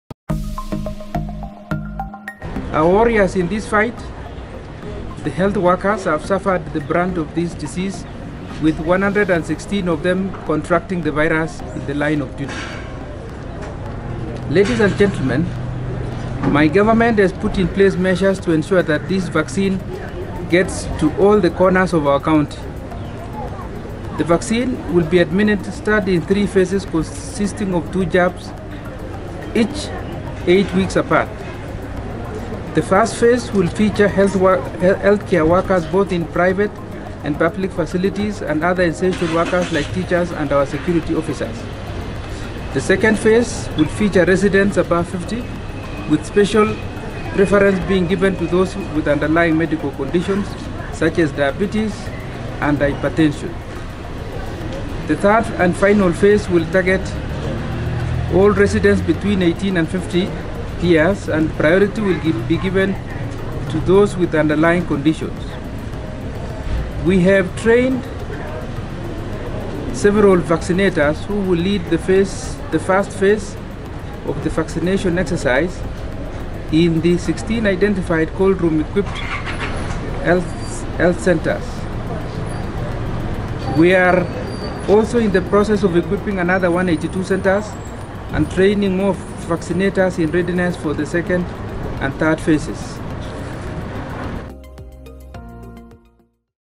Health workers in Kericho have began receiving Covid-19 vaccine
Speaking during the launch of the vaccination of first of the batch of the 3,000 frontline health workers, governor Paul Chepkwony said his administration had put in place measure to ensure that 6,000 doses gets to sub-counties counties in the county.

The vaccine will be administered in three phases which will be administered eight weeks apart. In the last one week, the County has recorded 15 positive cases. 12 out of them are currently admitted at the Kericho County Hospital. The county, has a cumulative 1,606 Covid-19 cases while 63 people have succumbed to the disease.

Video by Nikko Tanui